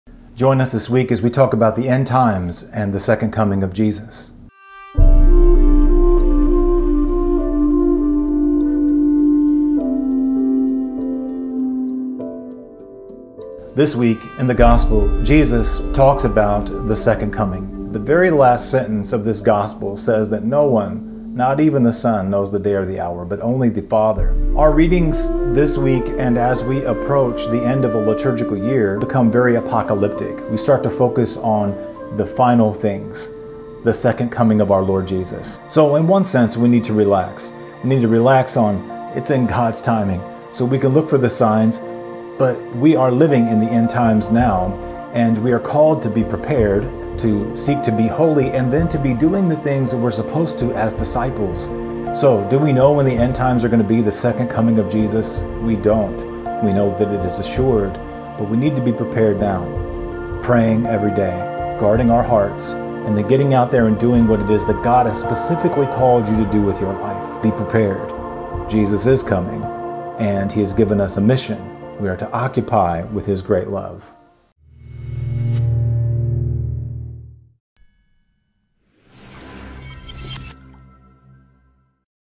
Weekly Catholic Gospel Reflection For November 18, 2018 | Thirty-Third Sunday of Ordinary Time
Jesus is coming and He has given us a mission. Father Burns reflects on the Gospel for the 33rd Sunday of Ordinary Time.

These weekly Gospel reflections are presented to you through a partnership with uCatholic and Spirit Juice Studios.